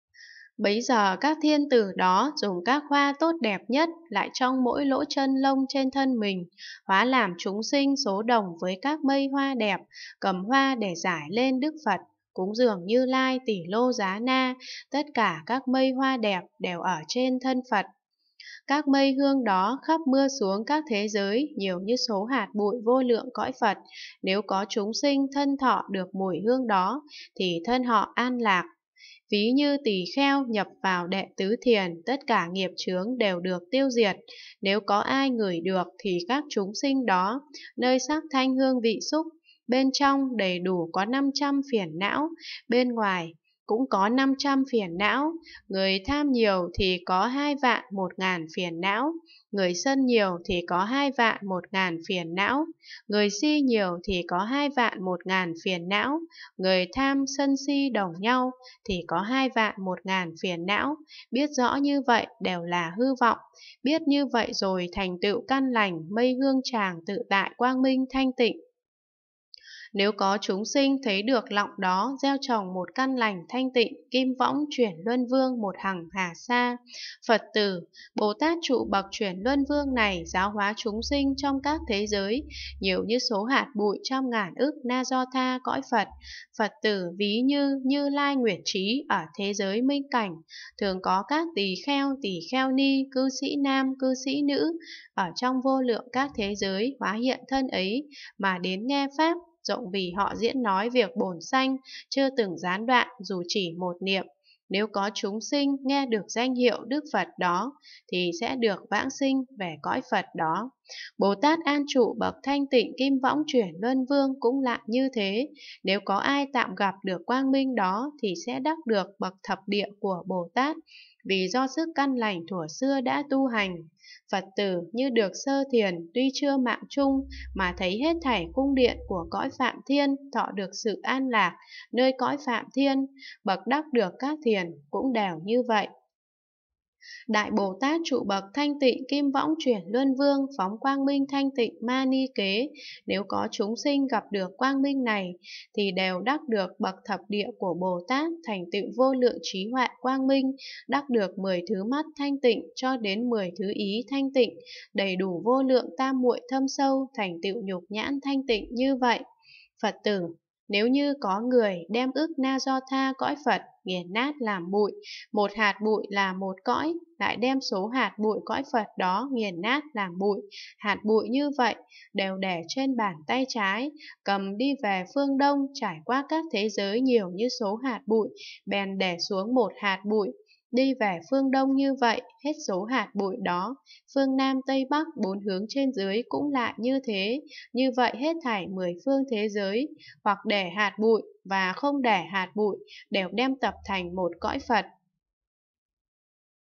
Tập 5: Kinh hoa nghiêm: Phẩm vẻ đẹp quang minh công đức của Như Lai thứ 35 - rất hay (100)
Phẩm vẻ đẹp quang minh công đức của Như Lai thứ 35